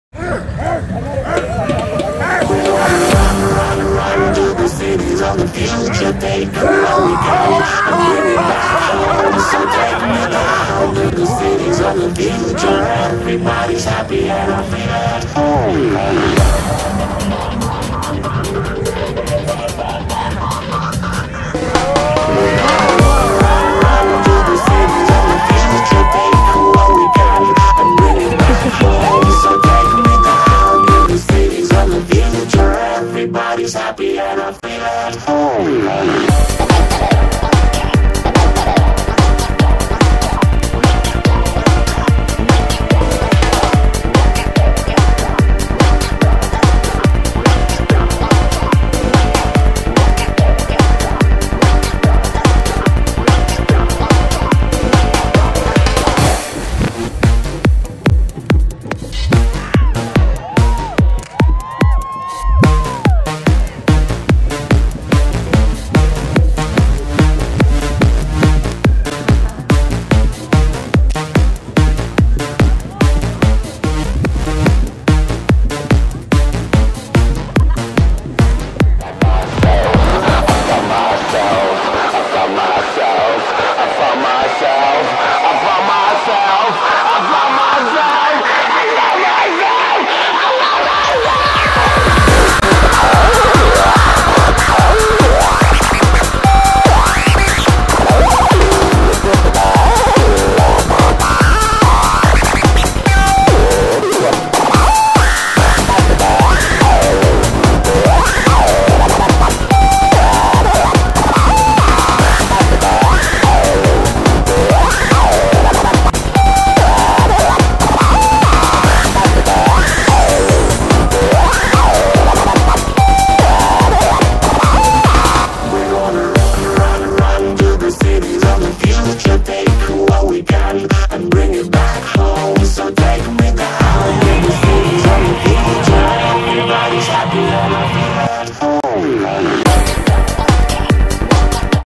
Apogaea 2009
apogaea preview in lake george colorado here's part of the fire show